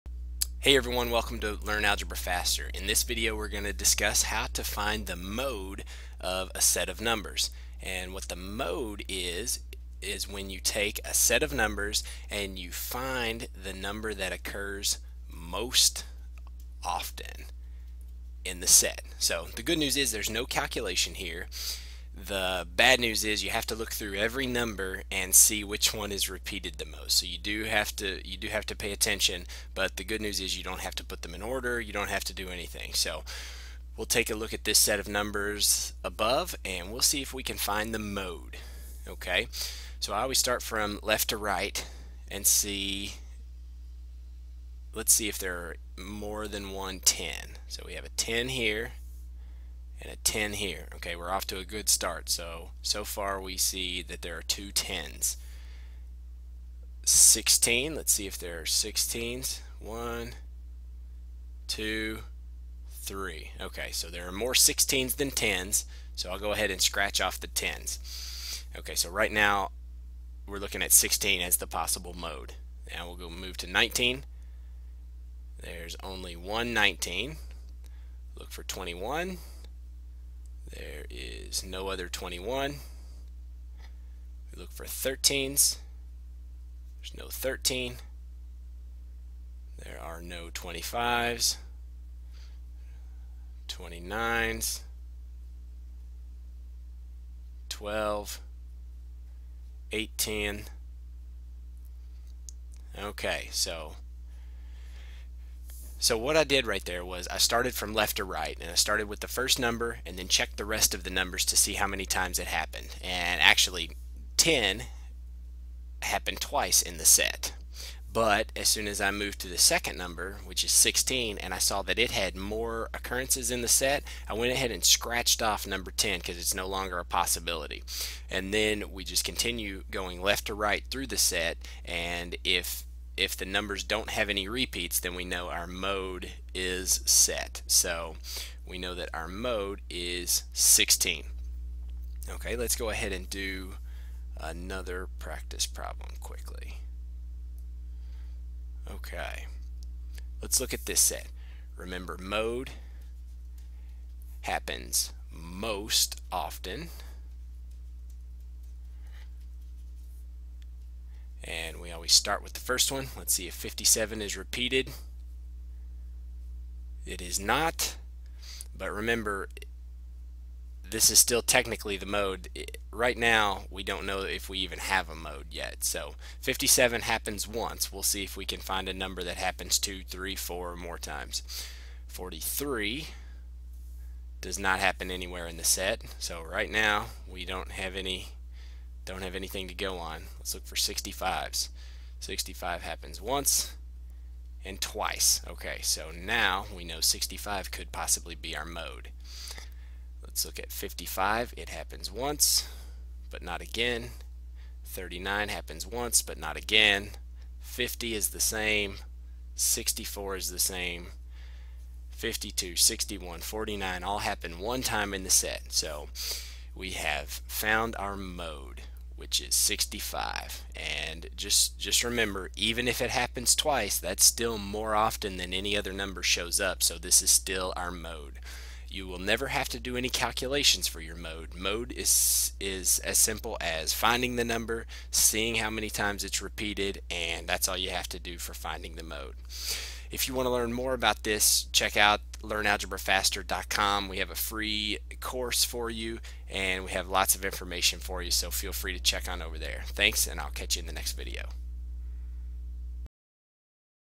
How to Find the Mode
Learn how to find the Mode of a Set of numbers. To get a transcript and learn more, head over